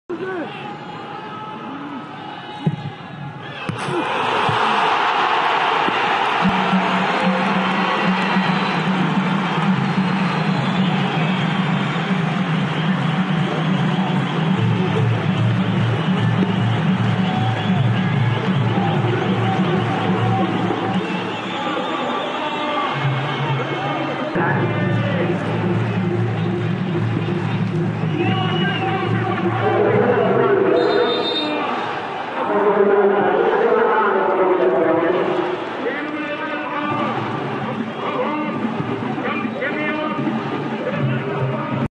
Al-FATEH - Ali Al Zaqan CLAQUE UN JOLI COUP FRANC
LDC Asie - Ali Al Zaqan a inscrit un magnifique coup franc ce mardi lors de la victoire d'Al Fateh face à Al Jazira (3-1), en Ligue des Champions d'Asie. L'attaquant de 25 ans avait auparavant inscrit le premier but de son équipe.

LDC Asia - Ali Al Zaqan scored a magnificent free-kick on Tuesday when Al Fateh triumphed against Al Jazira (3-1) in the Asian Champions League. The 25-year-old forward had previously scored his team's first goal.